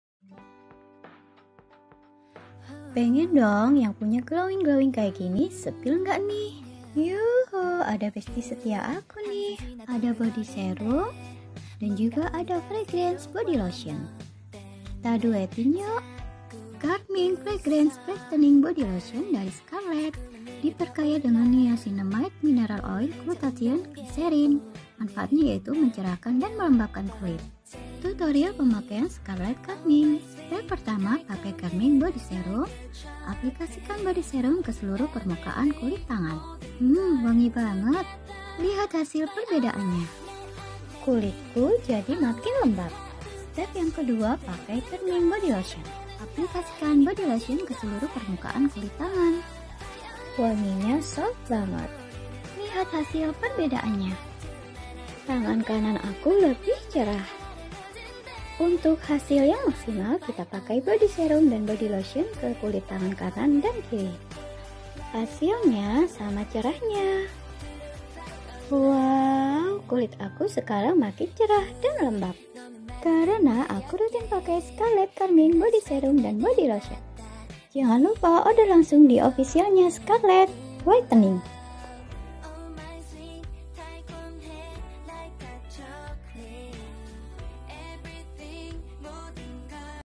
💜 Si Charming yg selalu I'm Coming 💜
Intip yokk Rahasia Si Charming💜
Worth it bgt sama Si Charming yg membuat aku I'm Coming🥰
@scarlett_whitening Body Serum dan Fragrance Brightening Body Lotion yang membuat kulit aku makin lembab dan cerah 😍
Wanginya suka bgt wangi Lavendernya💜

Selalu Excited bgt sama rangkaian produk skinker @scarlett_whitening 🥰
Apalagi nih The Best Varian Si Charming nya yg aroma wang-nya buat aku kesengsem😍

Body Serum dan Fragrance Body Lotion diperkaya dengan :
✨️ Niacinamide
✨️ Mineral Oil
✨️ Glutathione
✨️ Glycerin

💜 Melembabkan dan Mencerahkan Kulit

Kuyy sama'an yokk.. punya kulit glowing dan lembab😉
Dan sudah direkomendasiin langsung sama Ukhti Sidoarjo♡